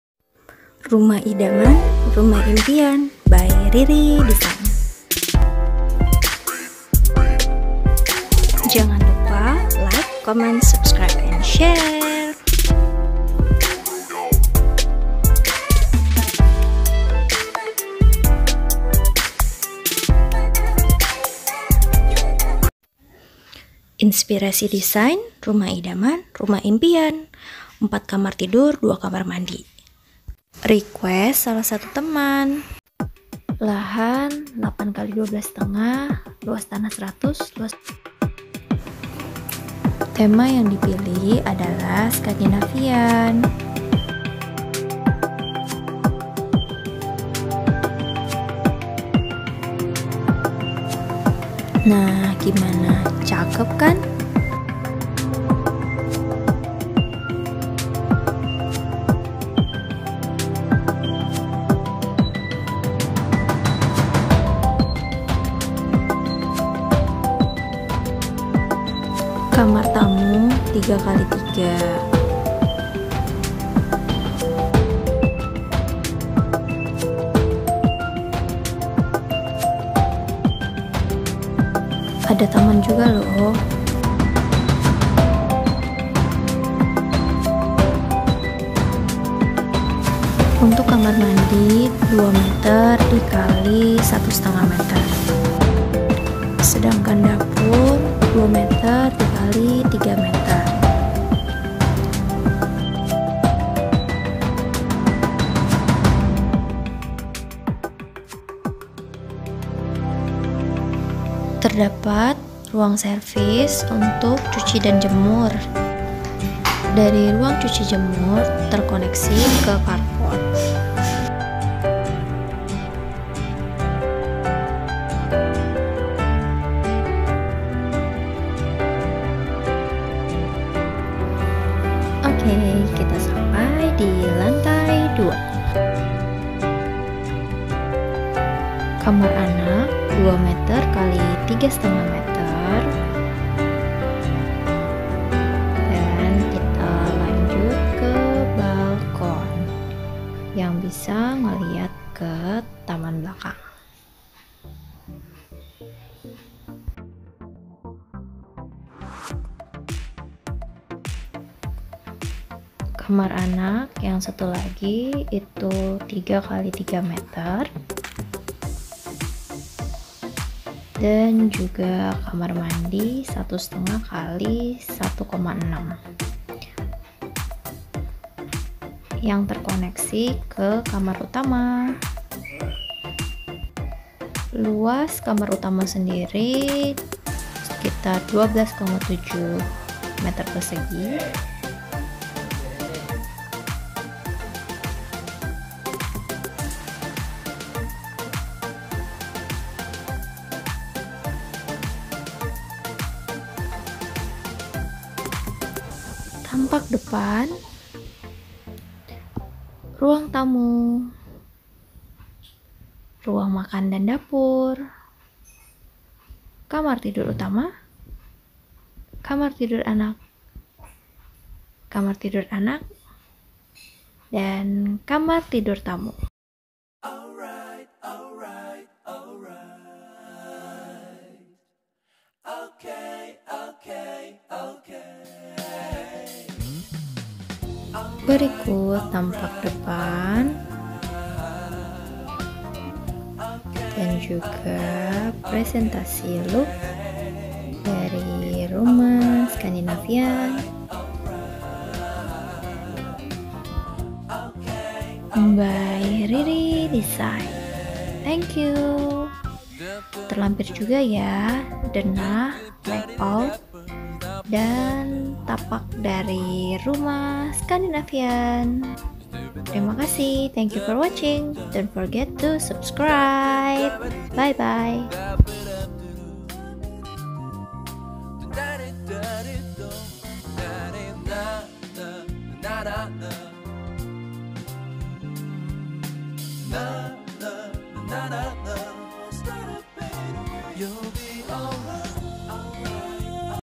Desain rumah minimalis scandinavian 4KT+2KM tipe 145/100 by RIRIdesign|| Rumah Idaman Rumah Impian
Jasa desain harga terjangkau. Cuma uang pulsa. Mulai dari denah, interior, eksterior + rekomendasi produk . Cek bio instagram @riridesign_